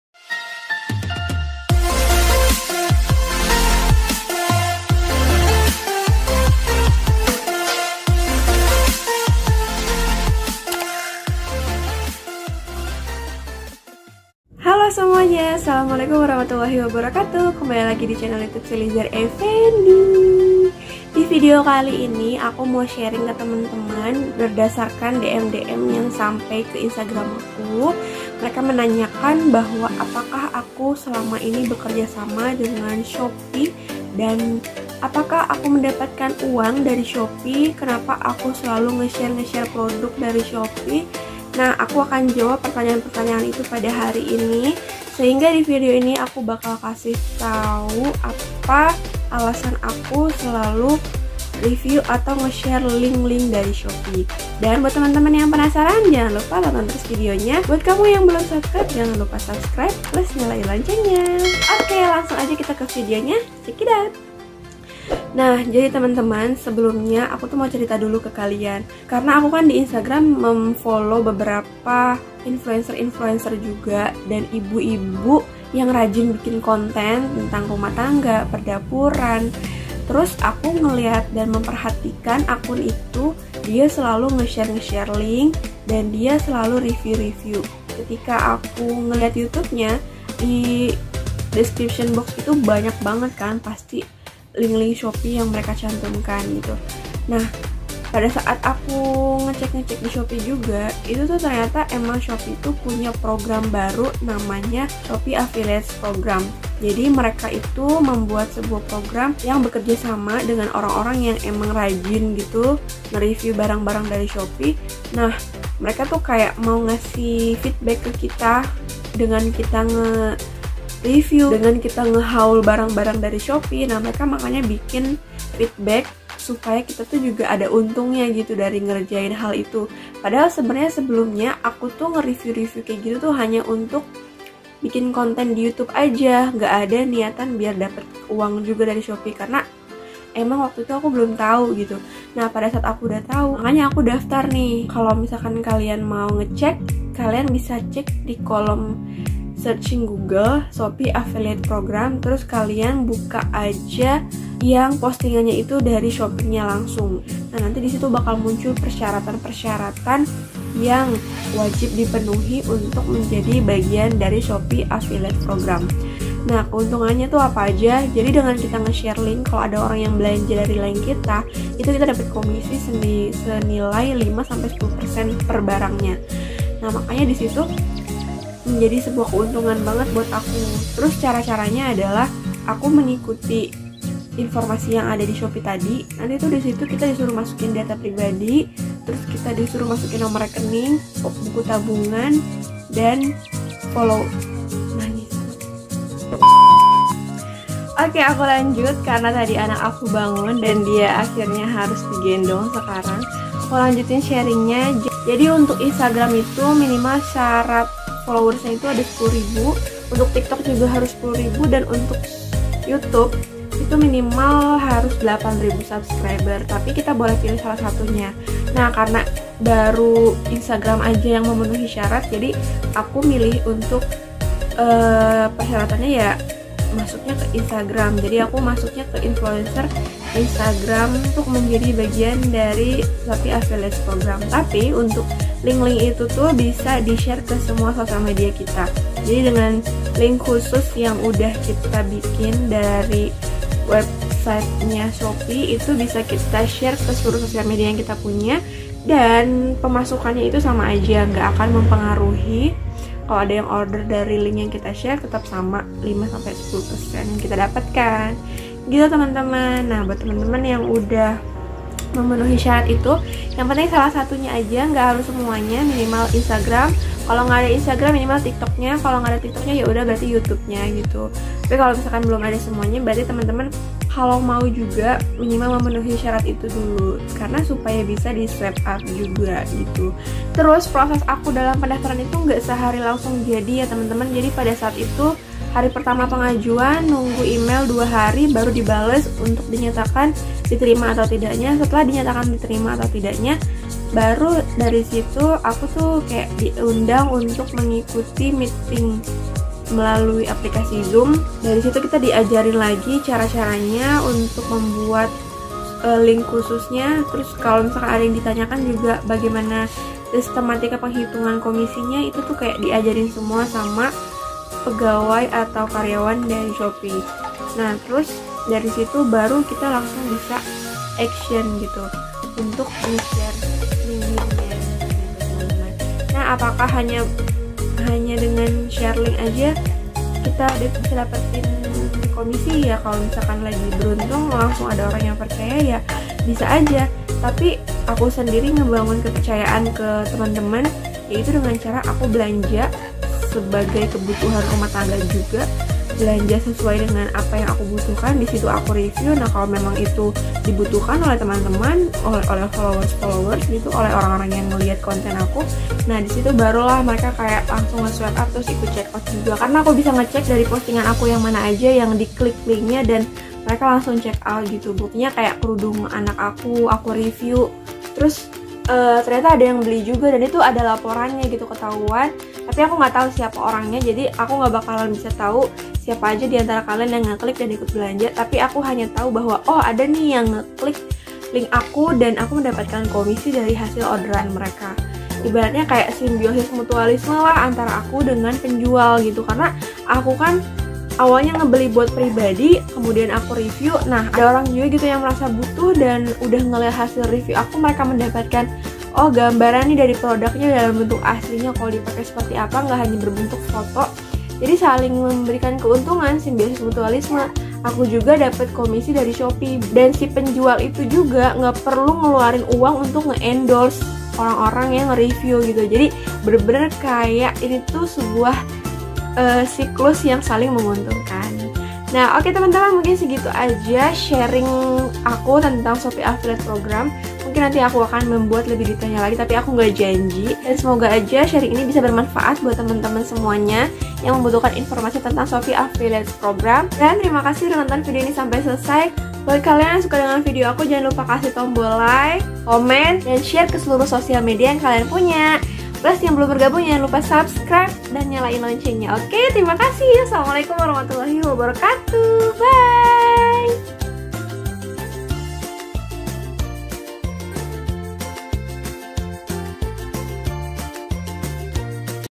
Sharing Shopee Affiliates Program, Beneran Dapet Cuan? Gimana Caranya?
shopee affiliate program
cara mendaftar shopee affiliates program

kamera :
Samsung A80

aplikasi editing :
Kinemaster

aplikasi edit thumbnail :
pixellab

Semoga bermanfaat, mohon maaf bila masih begitu banyak kekurangan.